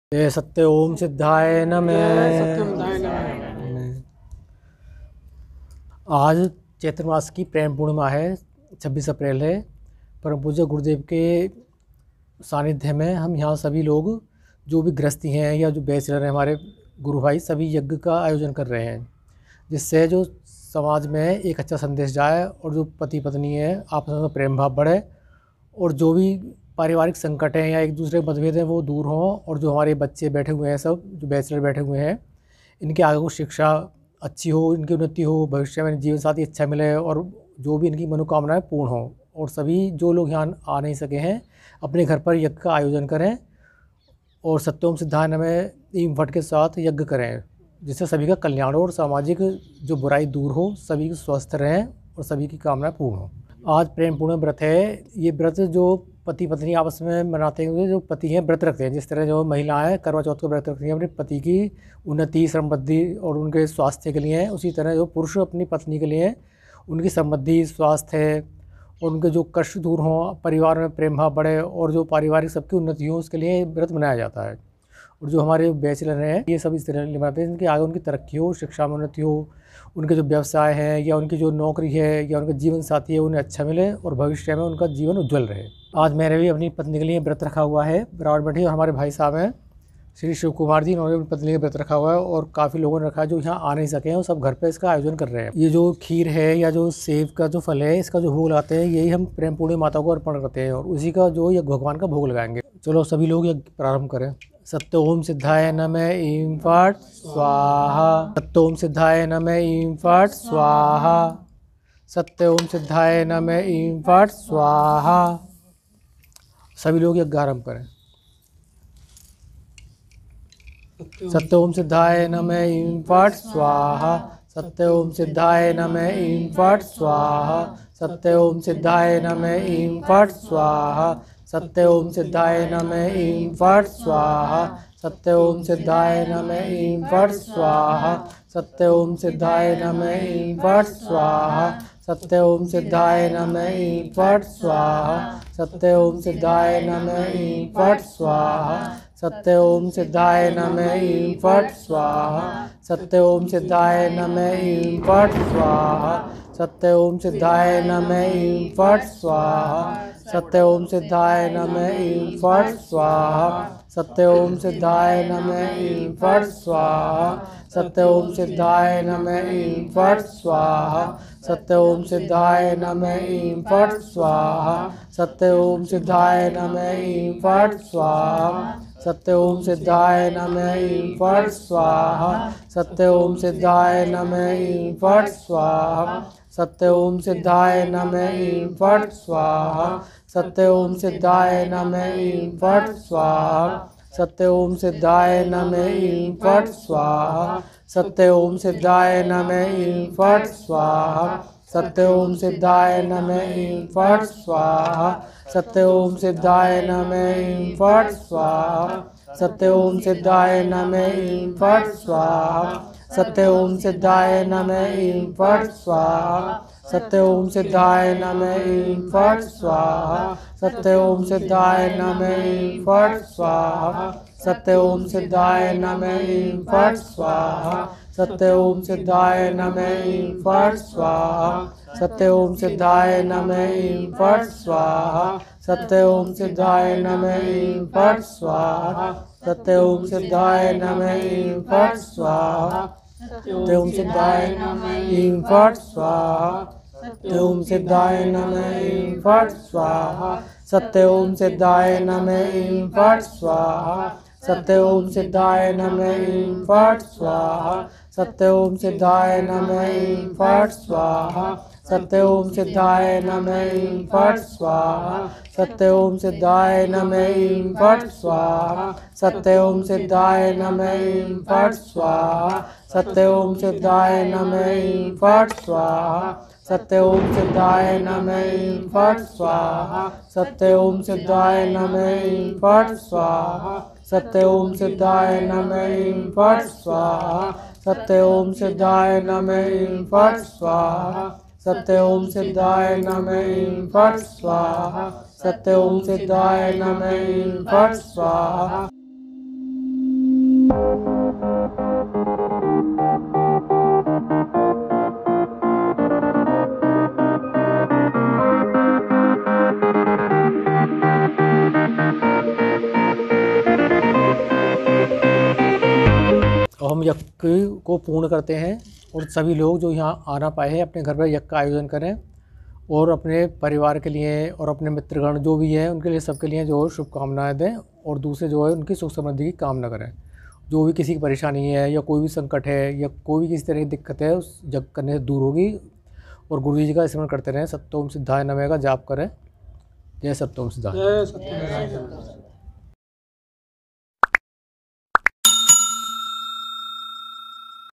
प्रेम पूर्णिमा व्रत दिवस पर महामारी ,रोग, दोष आदि से मुक्ति व प्रेम प्राप्ति के लिए किया गया यज्ञ
इस यज्ञ को देखे व मंत्र उच्चारण को सुने , साथ ही साथ अपने घर दुकान आदि पर इस वीडियो की आवाज जो सुने ओर हर तरह के रोग दोष से मुक्ति पाए